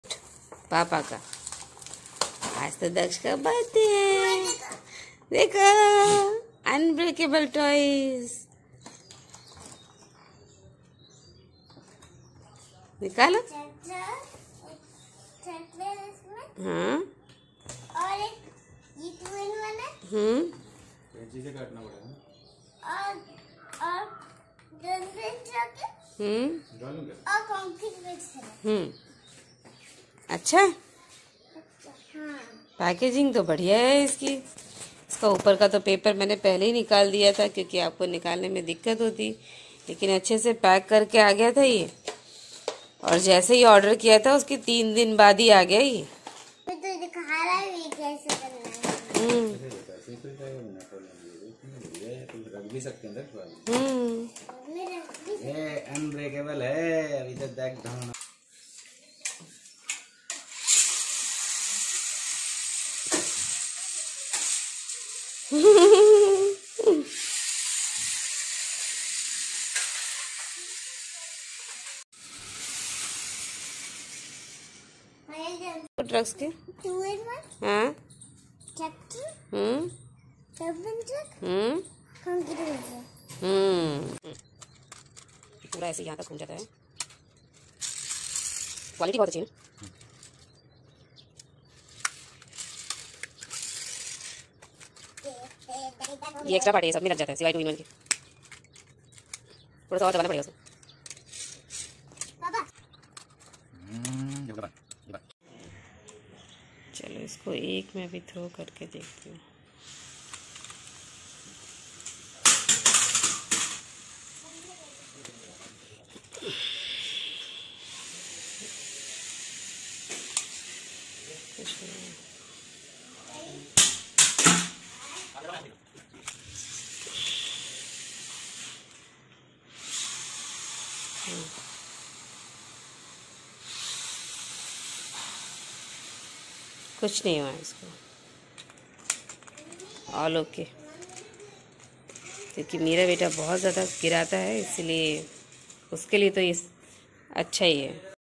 Toyshine unbreakable toys Unboxing, review and drop test
This is a video of unbreakable toys which is very popular among kids. This is an Unboxing and review of unbreakable toys truck from the brand of toyshine